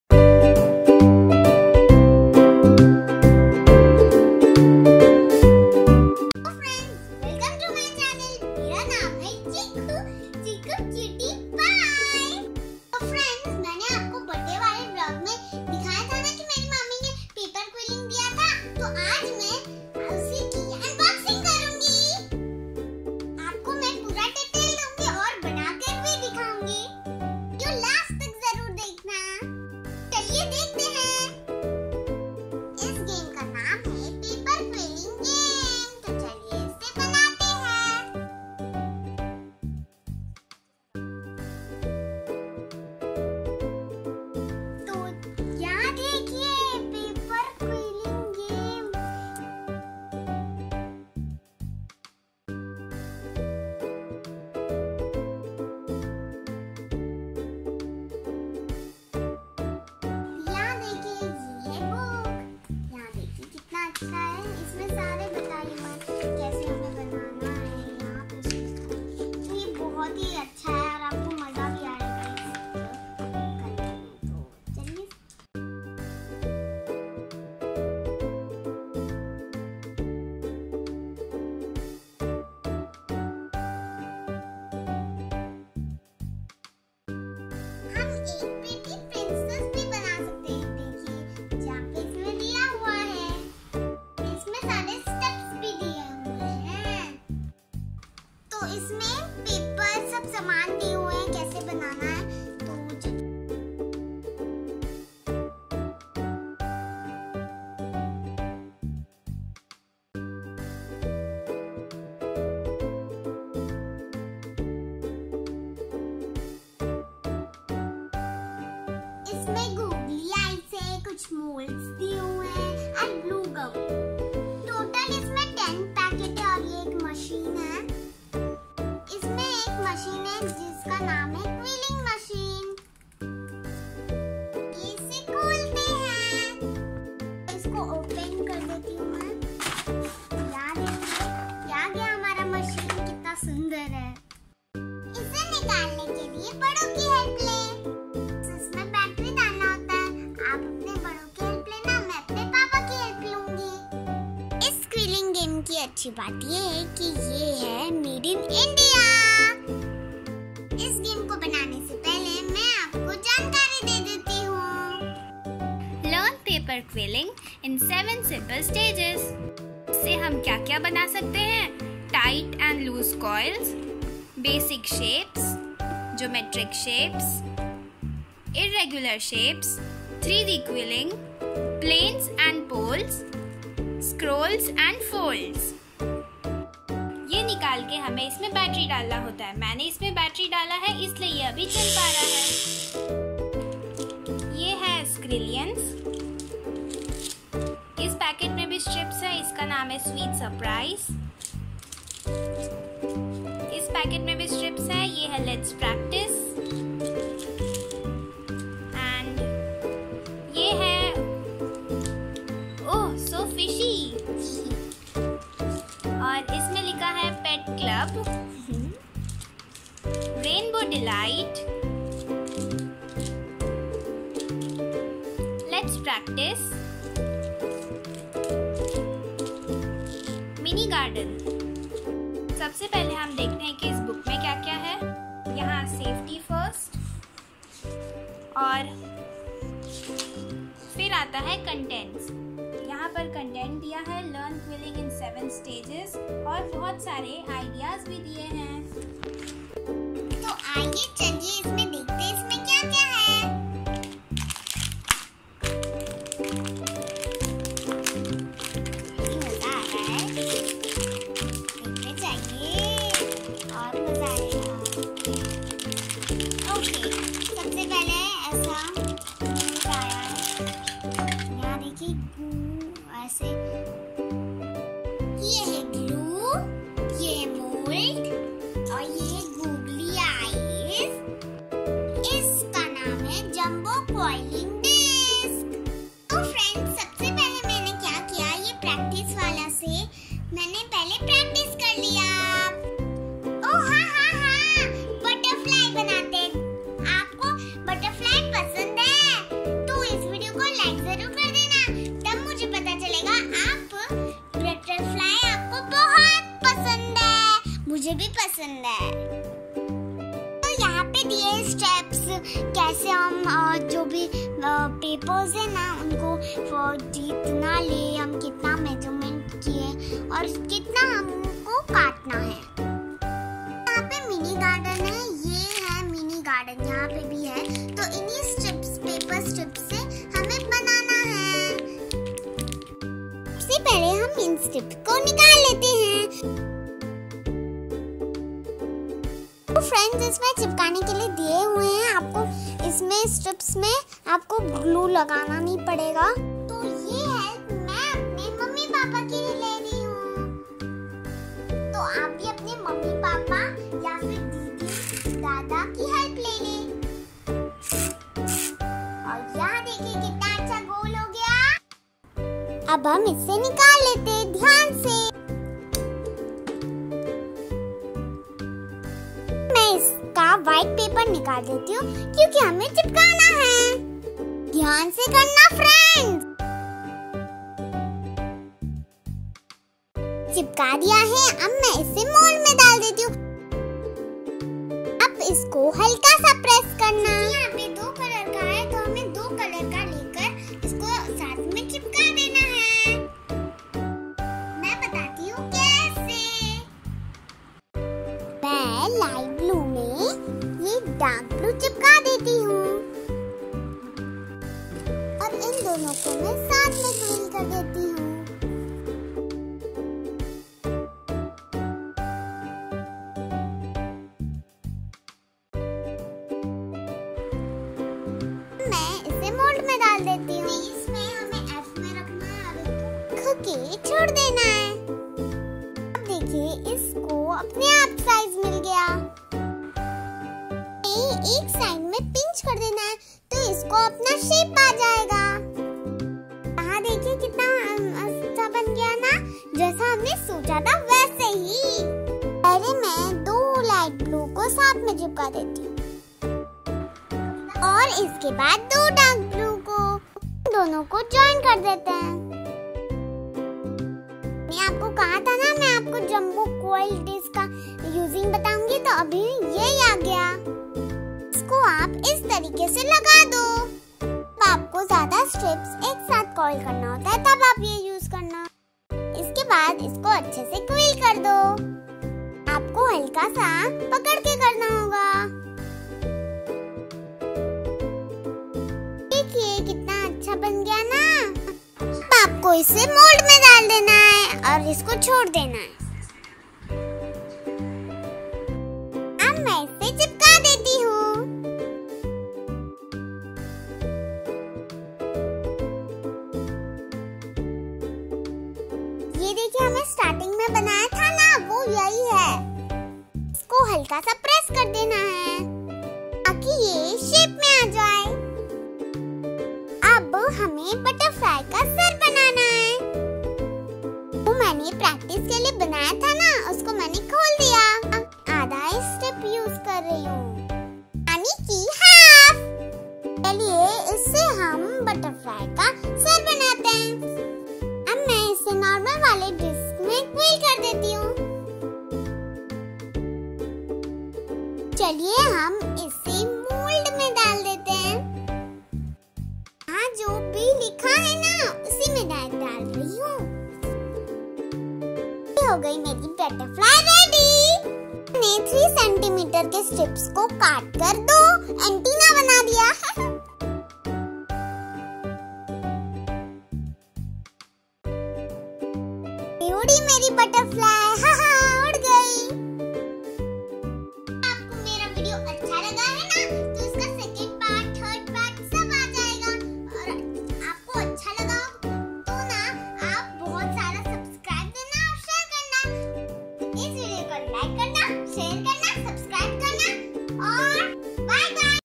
Paper Quilling Box | World Of Paper Quilling | UNBOXING + PLAYING!! | Chiku Cutie Pie
In this video, I am going to OPEN / UNBOX my WORLD OF PAPER QUILLING BOX!!

Do you know how MANY things we can make with it?

Watch the video till end and do try it at home!!
Let's be creative!!

Links for buying this ~

Here are the links to some of my videos~